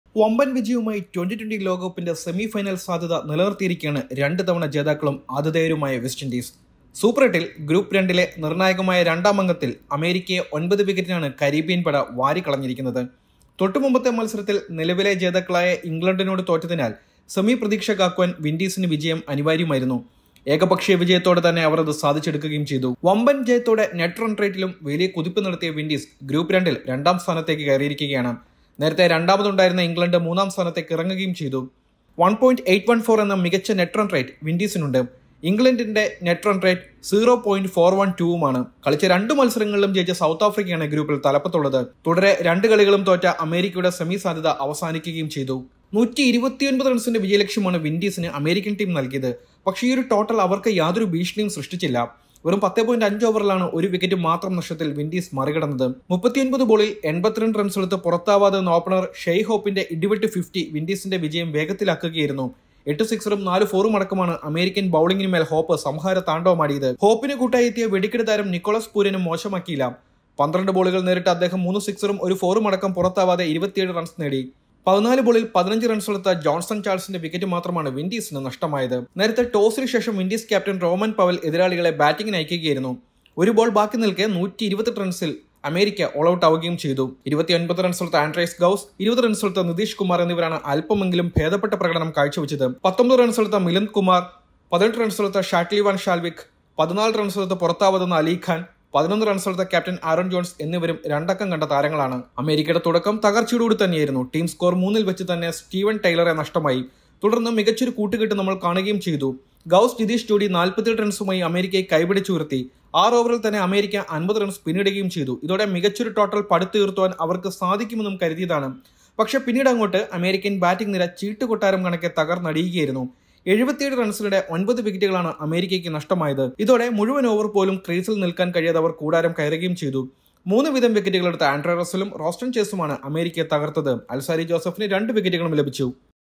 അമേരിക്കയെ നാണംകെടുത്തി കരീബിയന്‍ പട; ഇനി സെമിയിലേക്ക്
T20 World Cup 2024: West Indies Beat USA in Super 8 Clash | അമേരിക്കയെ നാണംകെടുത്തി കരീബിയന്‍ പട; ഇനി സെമിയിലേക്ക്
                ~HT.24~PR.272~ED.190~